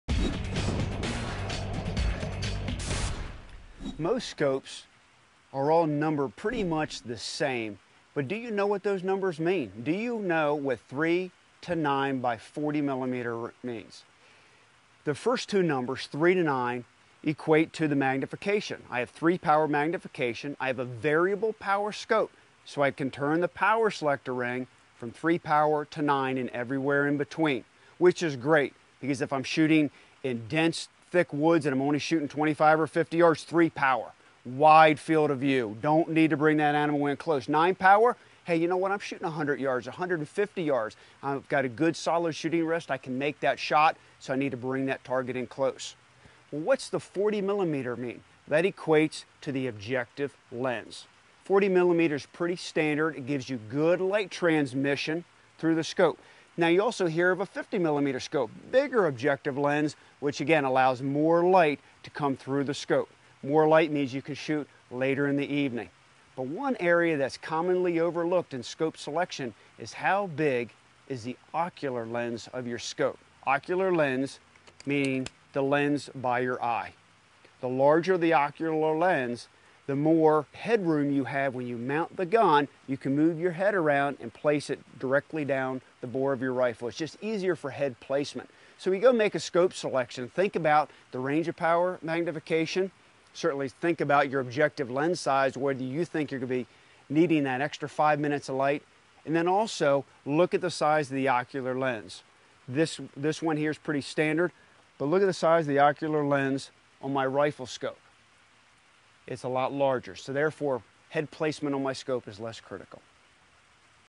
World of Muzzleloading: Scope Types
Unraveling the numbers associated with selecting the proper scope for your hunting application and weapon of choice.